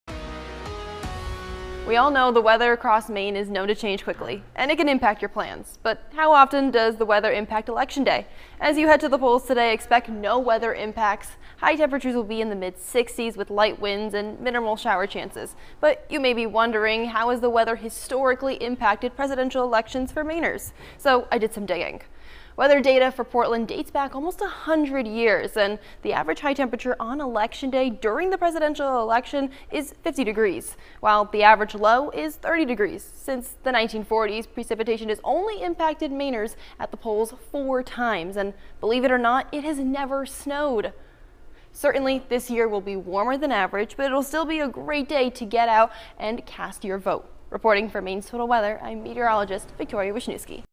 How weather has impacted Maine voters during previous elections
The weather in Maine can change quickly and impact plans, but how often does it impact Election Day?